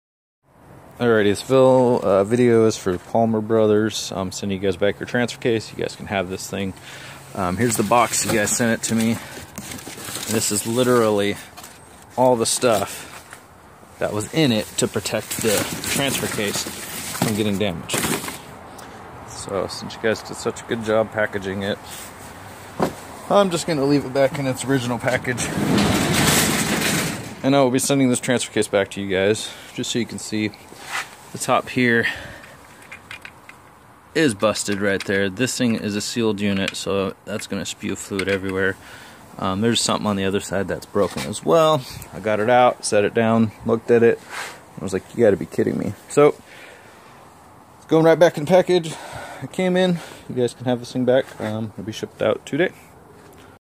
Broken transfer case as received 1
Showing packaging and repackaging of a transfer case I purchased off eBay